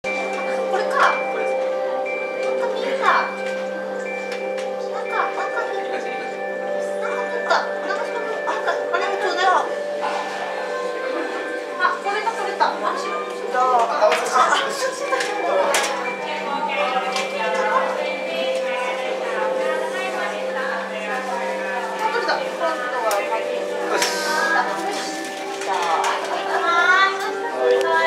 【スカイツリー展望台】天望回廊で落下物【マジックハンド】
東京スカイツリーのフロア450、天望回廊の外側（ガラス面）にデジカメのケース落下物があり、スタッフがマジックハンドで引き上げていました。
撮影に夢中になり、物が落ちることがあるようです。